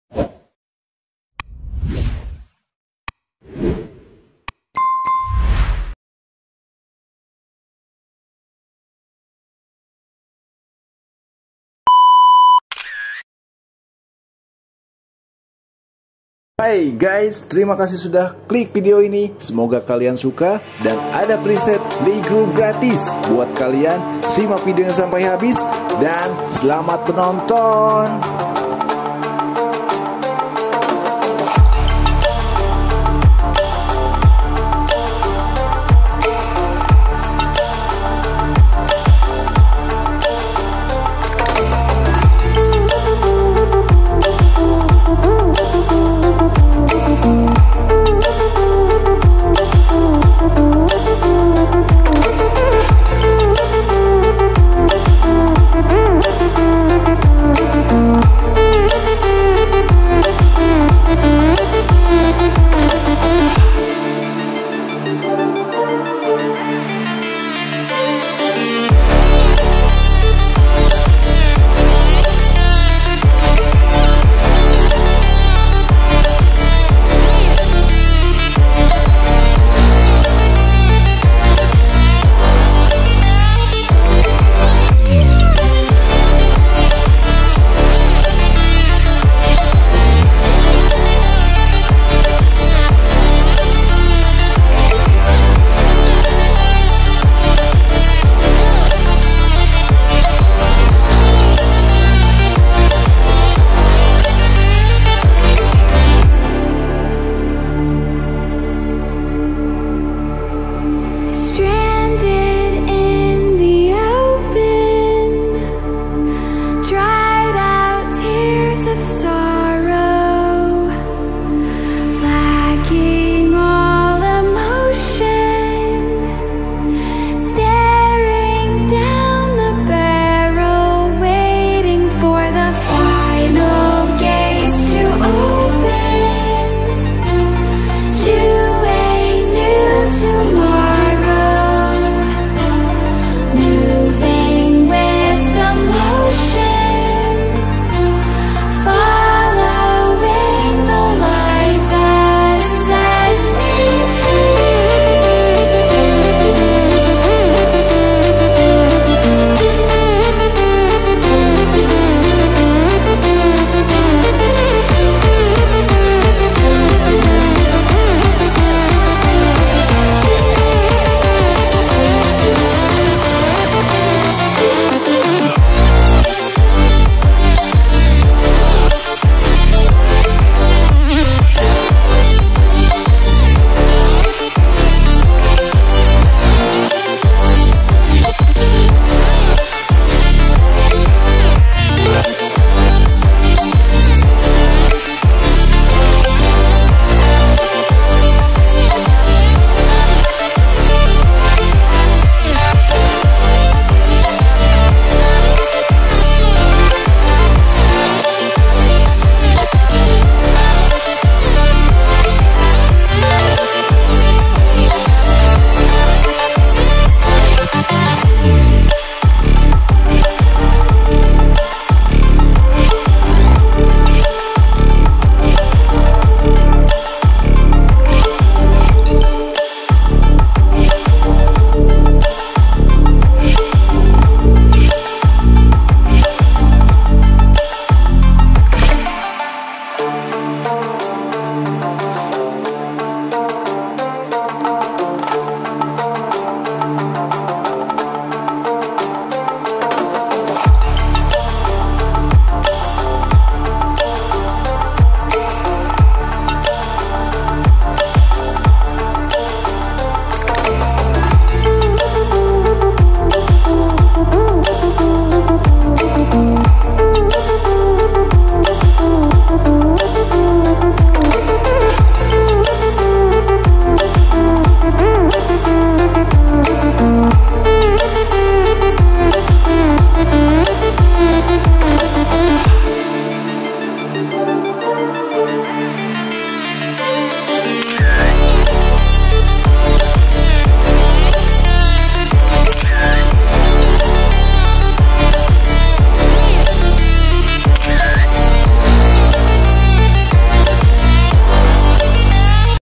Tutorial edit foto di Lightroom , Free Preset Lightroom cerah aestetic
FREE 50+ PRESET LIGHTROOM | KULIT CERAH AESTHETIC | LIGHTROOM TUTORIAL

Preset Lightroom Yang Akan Aku Bagikan di Video Kali Ini Adalah Preset Lightroom Kulit Cerah Aesthetic.

Untuk Mendapatkan Hasil Yang Maksimal Sesuaikan Lagi Pencahayaan Nya Dengan Keadaan Foto Kalian Ya.

Preset Untuk Pengguna Android ⬇️
📌 Link Google Drive

Semua Foto Yang Di tampilkan Di Dalam Video Adalah Milik Pemiliknya Masing-Nasing. Semua Foto Hanya Untuk Tujuan Ilustrasi.